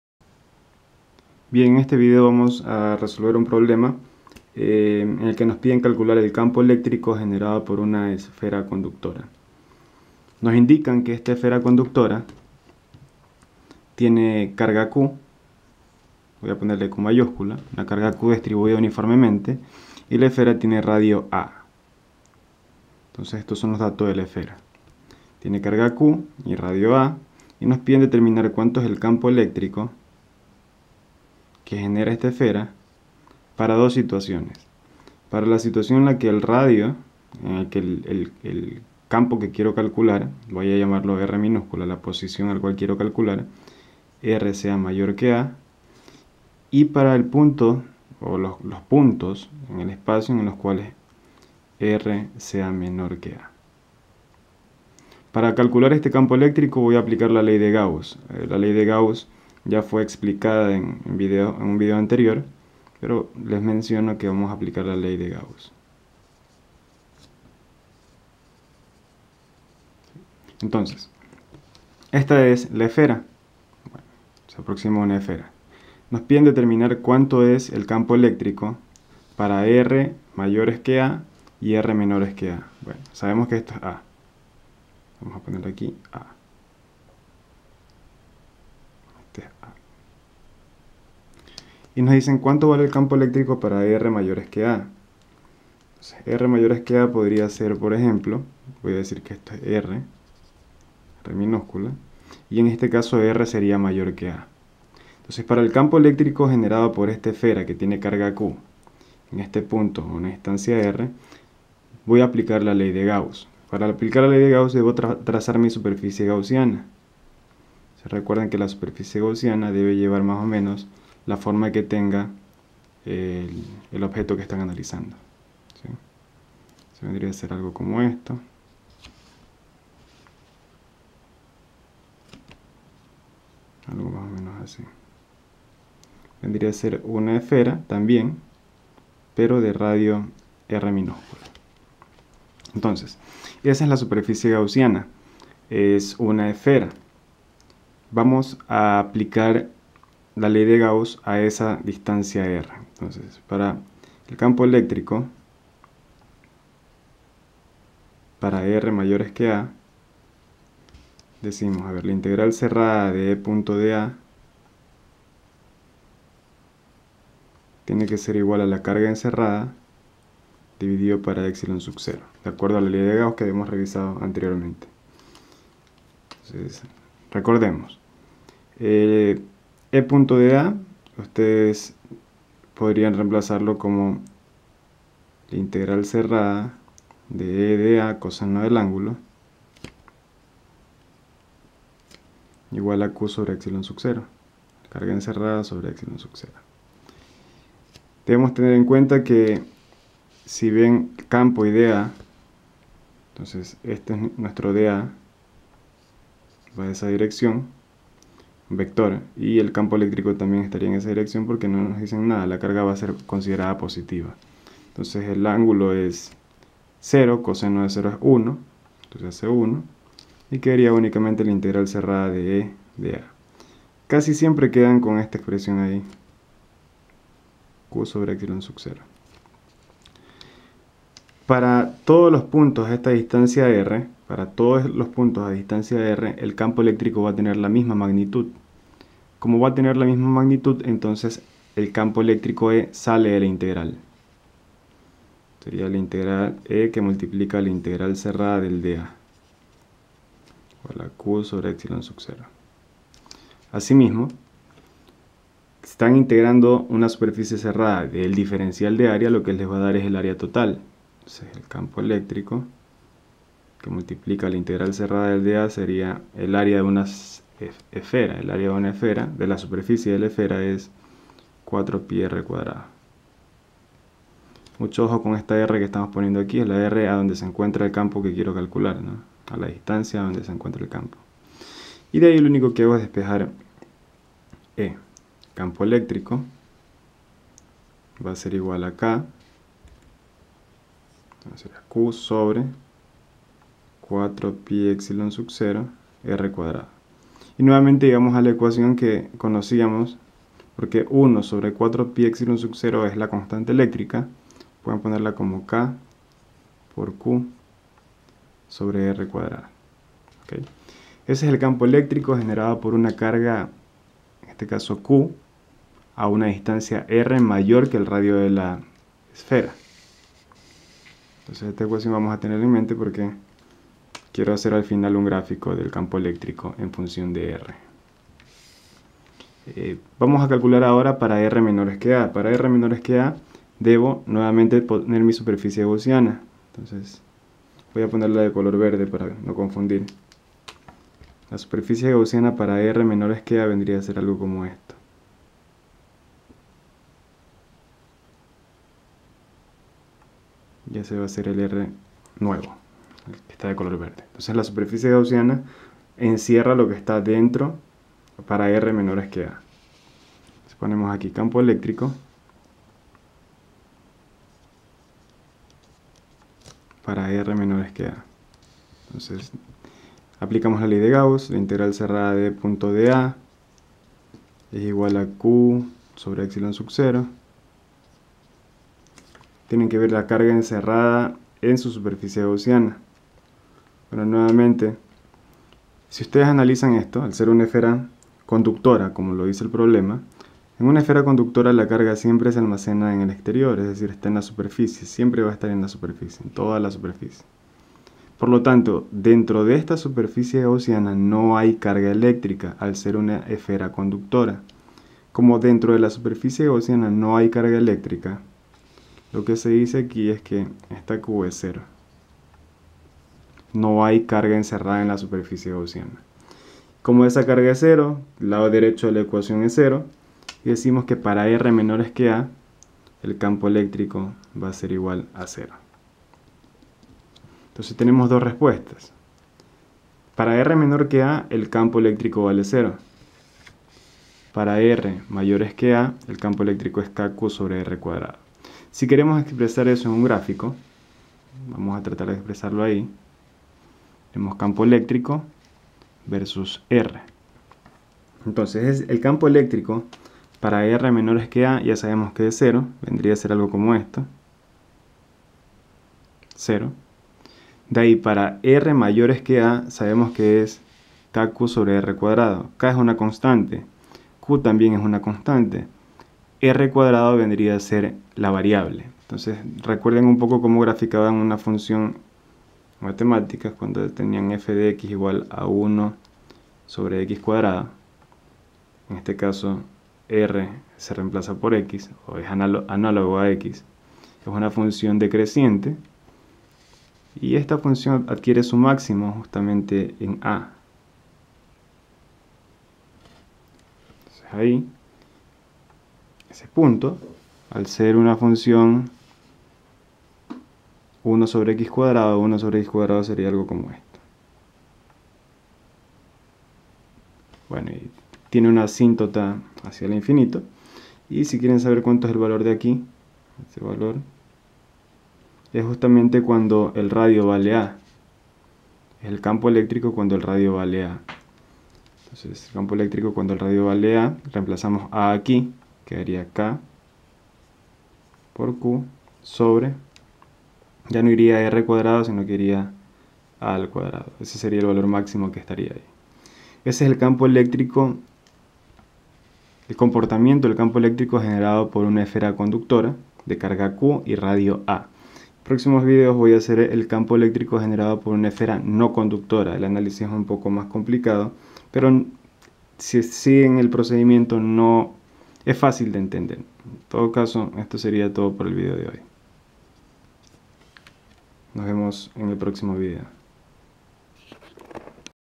Campo eléctrico generado por una esfera conductora - Aplicación de la Ley de Gauss
Ejercicio - Aplicación de la Ley de Gauss en el cálculo del campo eléctrico generado por una esfera conductora  que tiene carga Q y radio a. Campo eléctrico para regiones dentro y fuera de la esfera conductora.